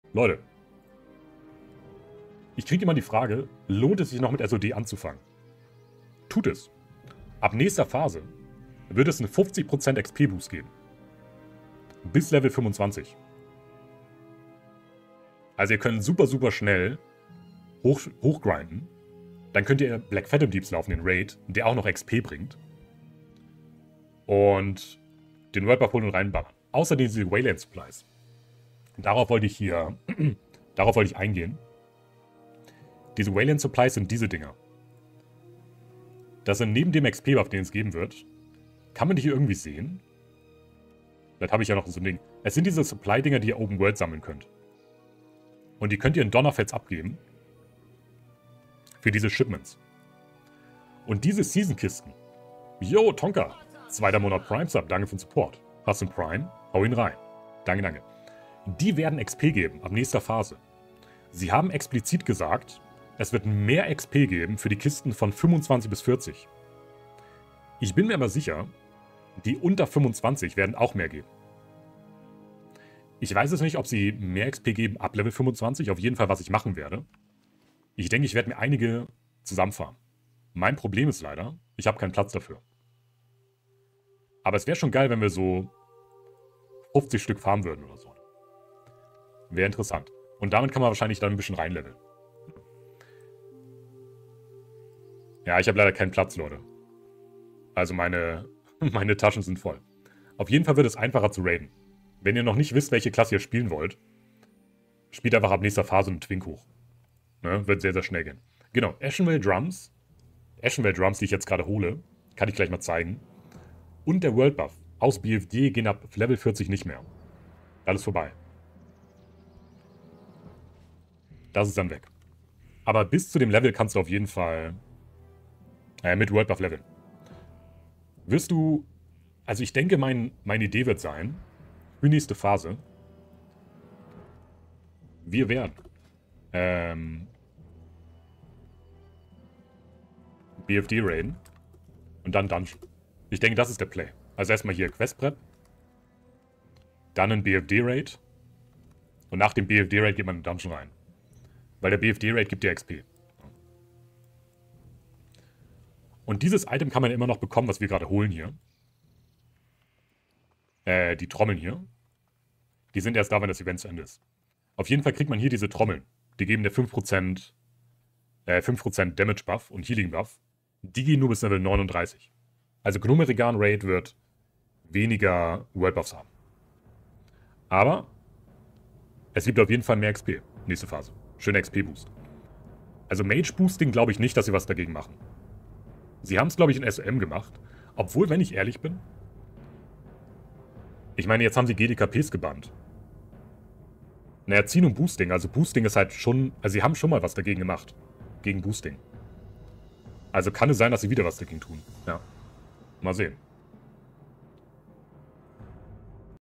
XP Boost in Season of Discovery und Item Änderung für Zusatz XP in Phase 2 Preview
Classic + plus und Season of Discovery. SoD Release

Neben WoW gibt es auch andere Spiele und Gaming Clips und Gaming News zu MMOs